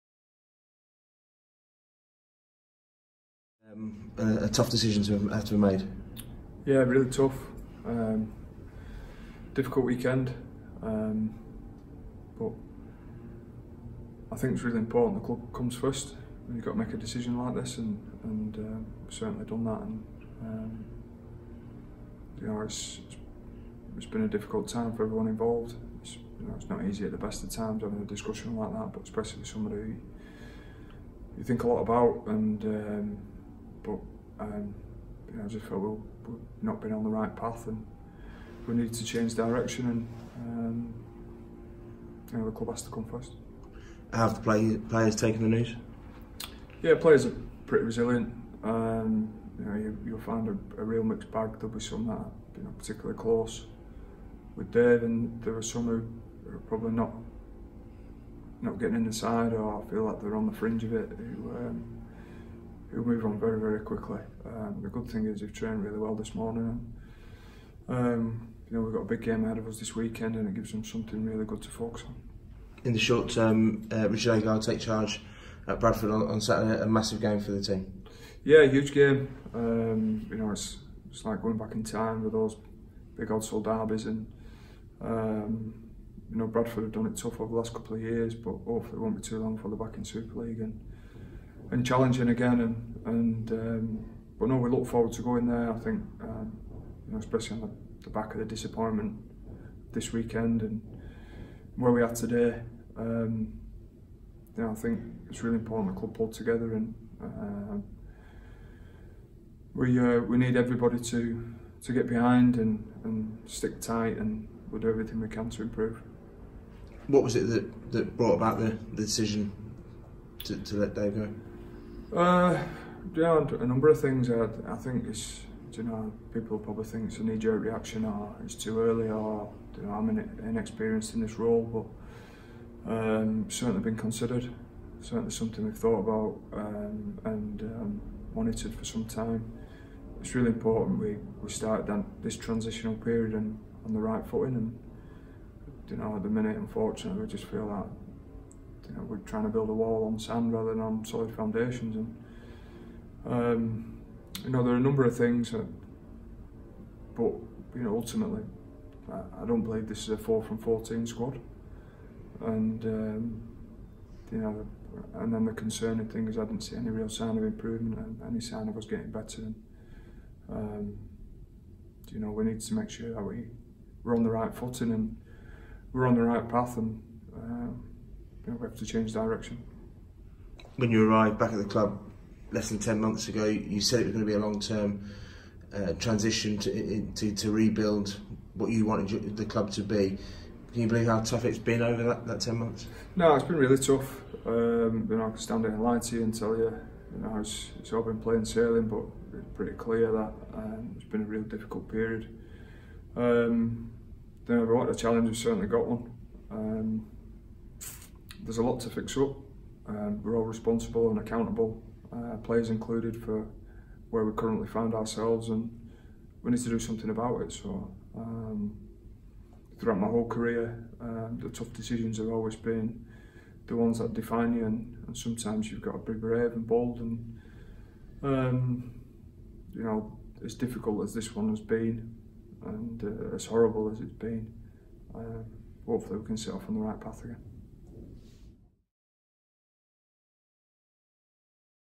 Sinfield talks about coaching decision
Leeds Rhinos Director of Rugby Kevin Sinfield gave his reaction following the news today that Head Coach David Furner had left the club.

Sinfield admitted it had been one of the toughest decisions of his career but said the decision was taken so to give the club the best chance of building solid foundations for the future growth of the team.